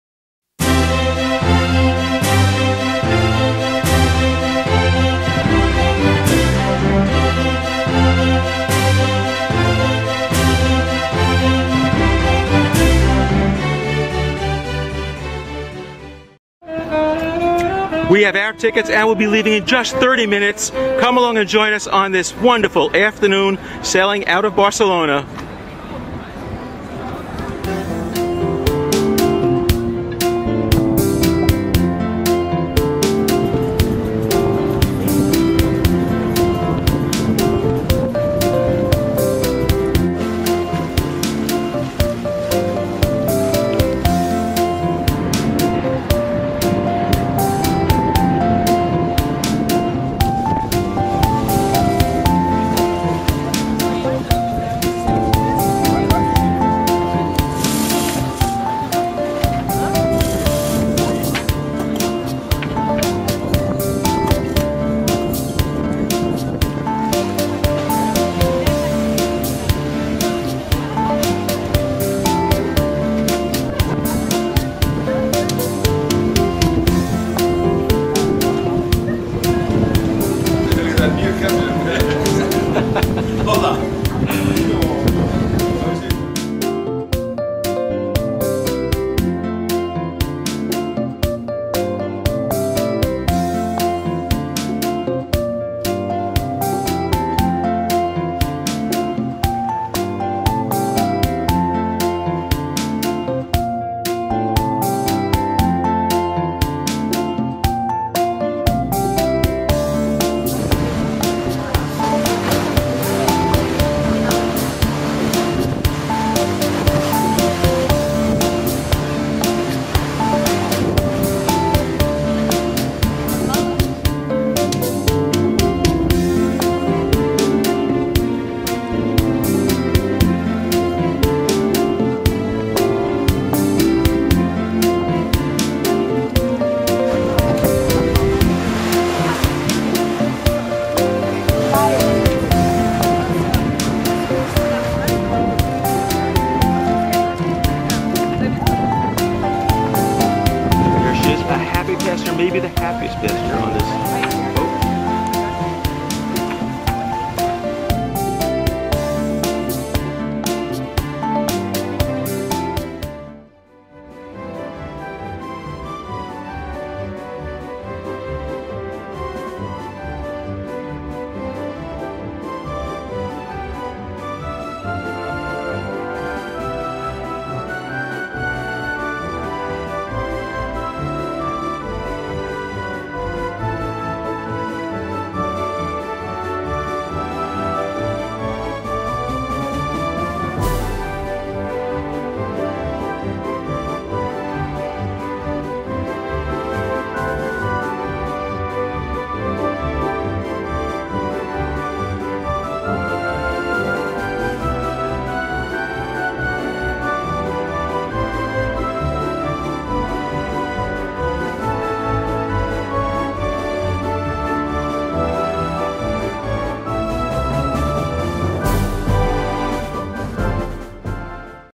BARCELONA'S BEST CATAMARAN SAIL - THE AWESOME ORSOM
We enjoyed  the ultimate Barcelona sea, sun, fun, sailing day plus live 'jazz' tunes. Join us on our 90 minutes ORSOM afternoon coastline sail out of Barcelona harbor.

At about 13 euros/adult this is also hands down the best sailing bargain in Barcelona town.

You may email, call or visit Orsom's harbor kiosk for public sailing schedules and to purchase tickets. The schedule is issued each Friday for the following week. For more details here is Orsom's web site: